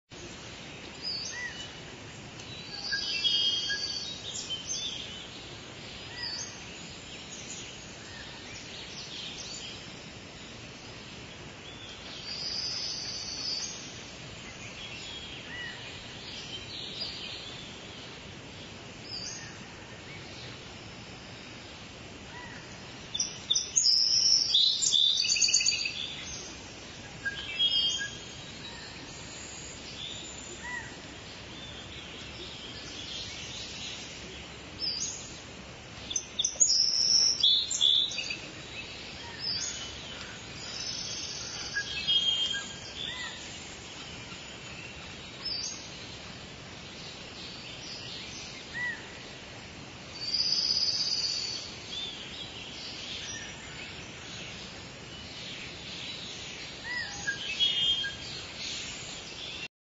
Rainforest Ambiance | Sound Effects HD
The mood of the rainforest with bird and cricket sounds